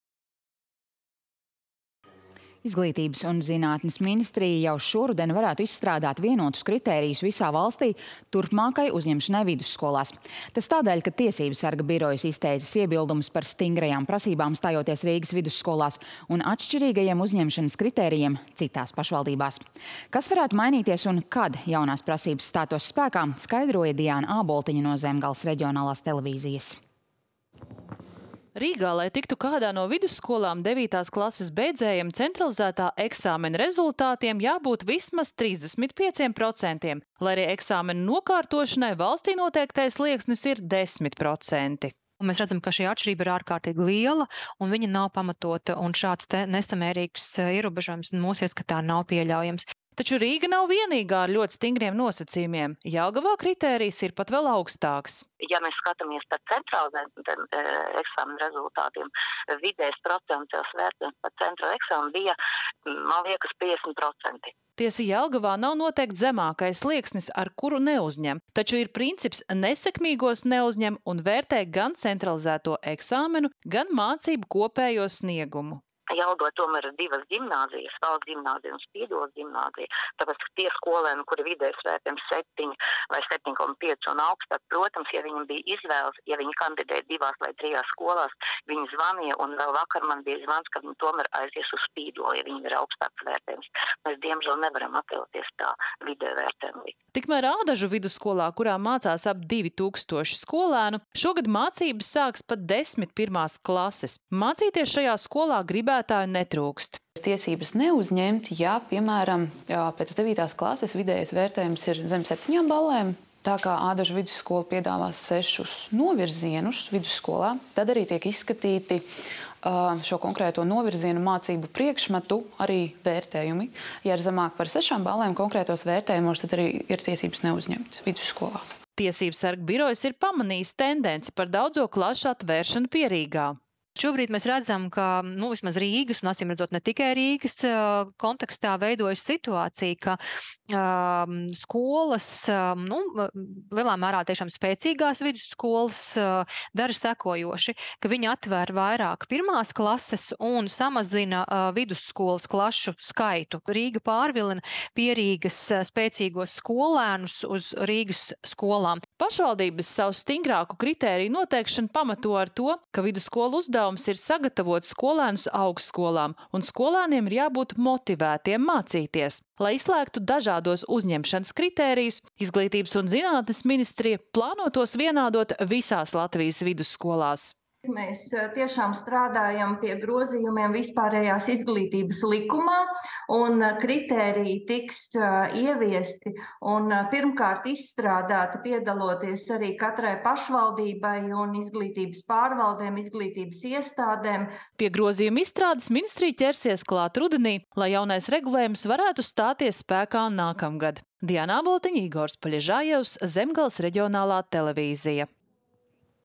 Vienādos iestāšanās kritērijus Latvijas vidusskolās
Ņemot vērā Tiesībsarga iebildumus par stingrajām prasībām, stājoties Rīgas vidusskolās, kā arī atšķirīgos uzņemšanas kritērijus arī citās pašvaldībās, Izglītības un zinātnes ministrija jau šoruden plāno izstrādāt vienotus kritērijus visā valstī turpmākai uzņemšanai vidusskolās. Turpina Diāna Āboltiņa no Zemgales Reģionālās televīzijas.